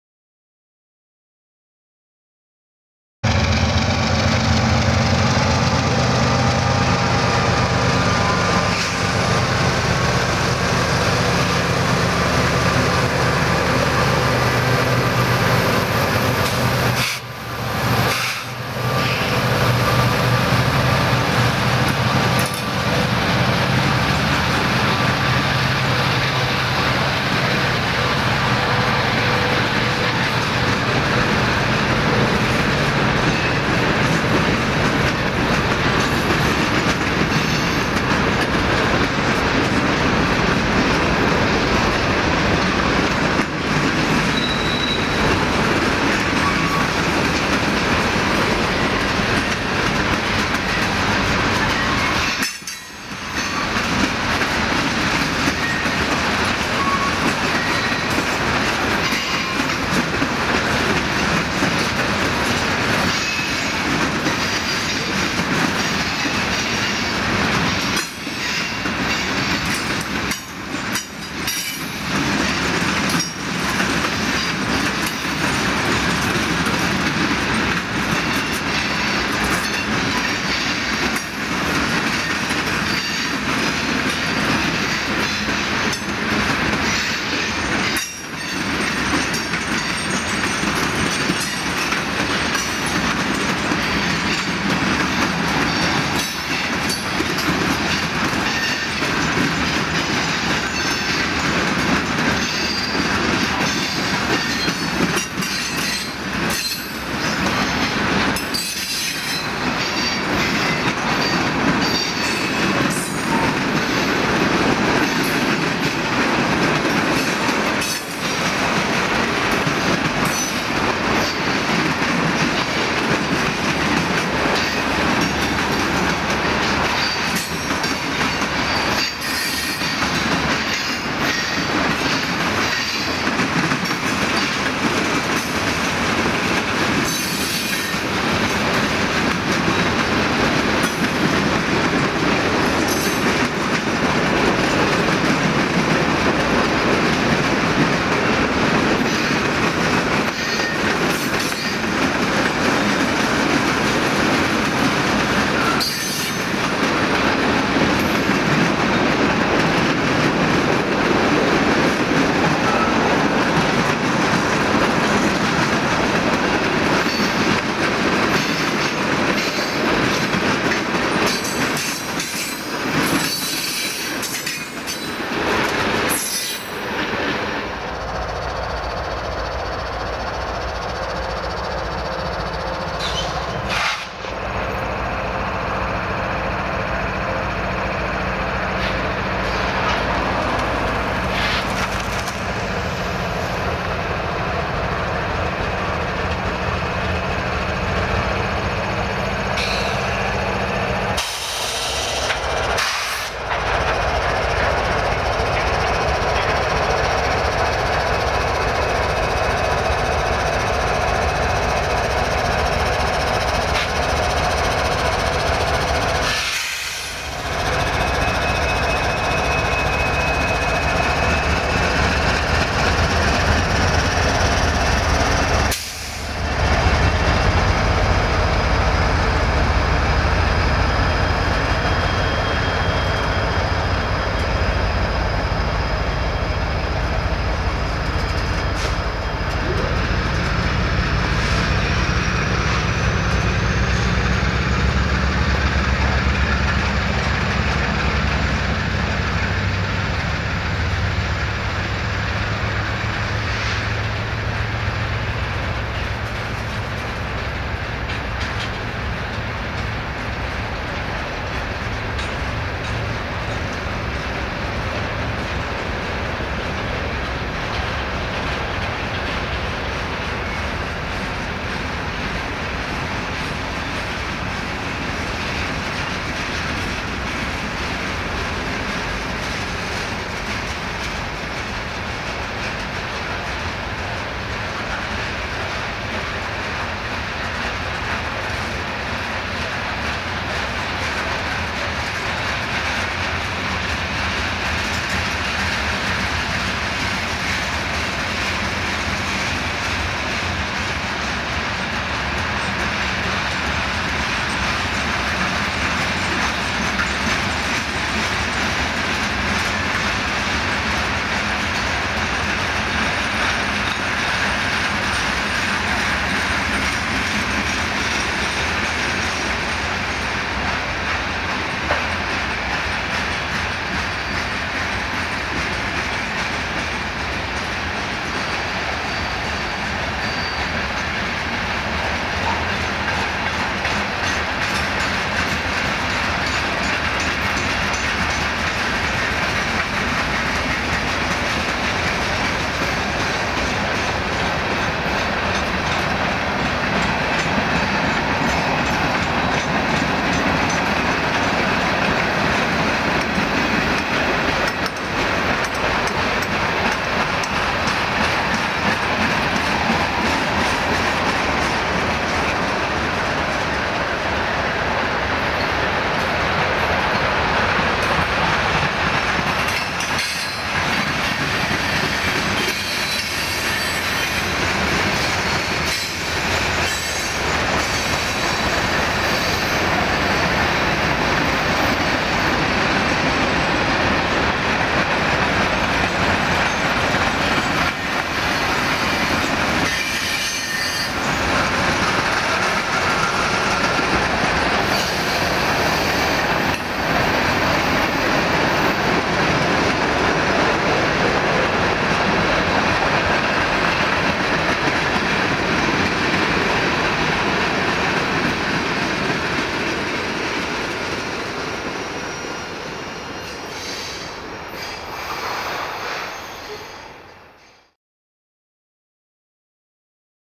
The Same Train Twice
I caught this train during the day and caught it again as I was heading home. I've always loved the silhouette of a train as it passes in the dark!